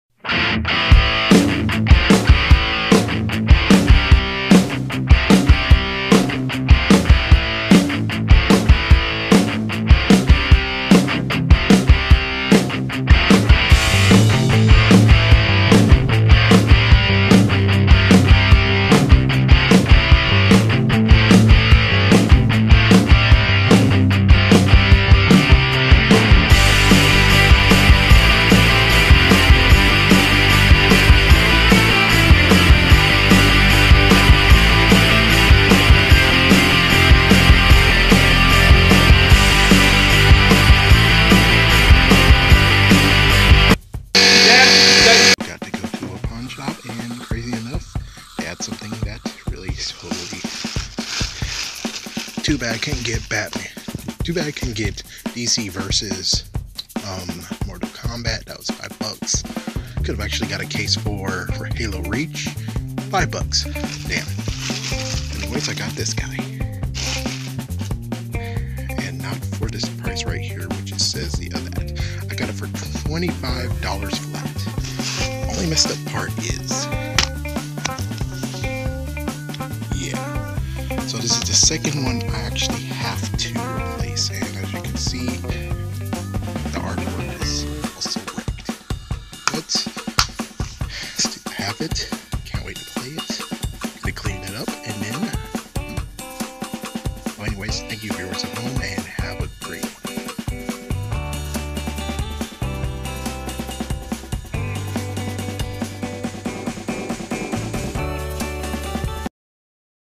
Quick Pawn Shop Grab #5: Snag a Wii U Top List Game NOW
So, went to the pawn shop that is close to a place. It had some nice grabs but I blow it all for one pick. I just wanted to see the condition and well he was smart to say it is $25. So... of course!

Why get Halo Reach for $5 if I got a copy for $1. Sadly, it is only a disk and case no cover or manual.